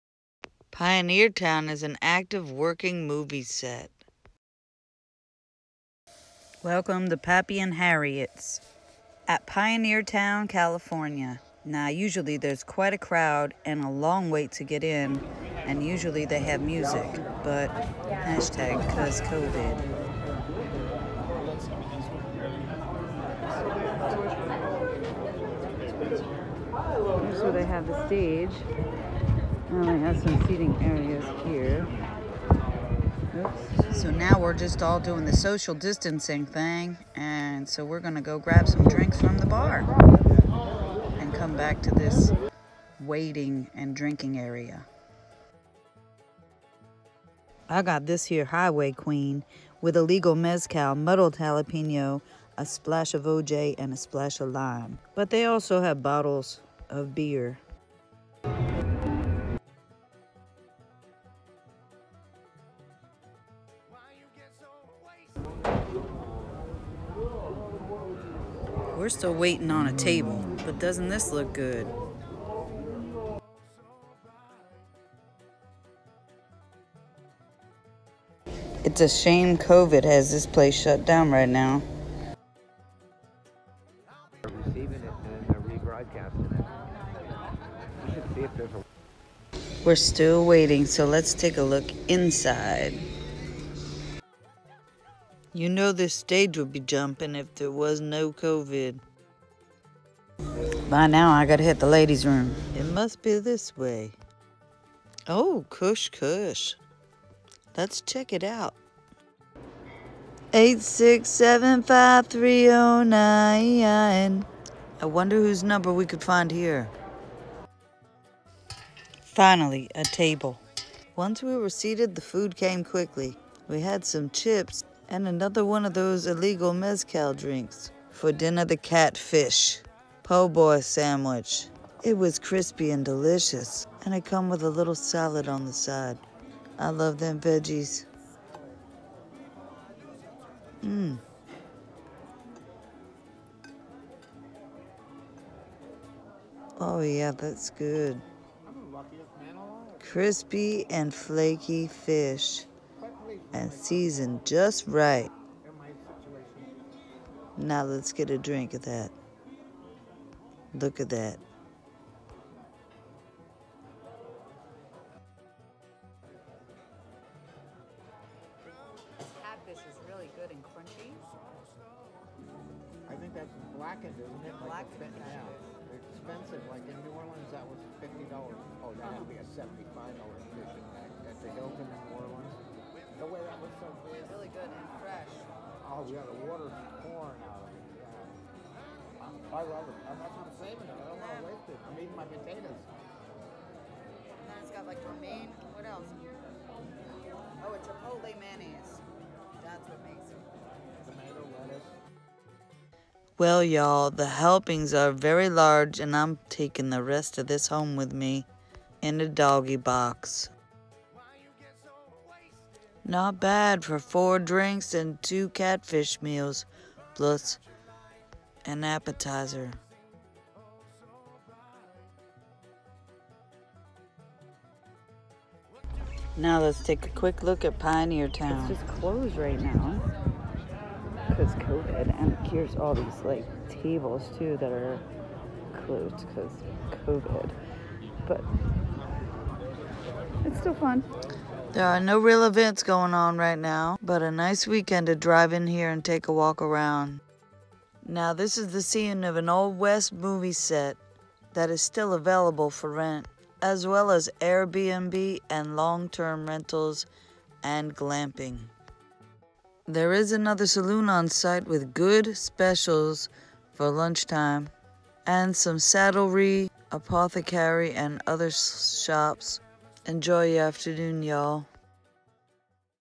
Pappy and Harriet’s Saloon Stage & Pioneertown Old West Movie Set
Bob, Robb, Liz and I go for a bite and a cocktail at Pappy and Harriets, And visit the old west movie set of Pioneertown💜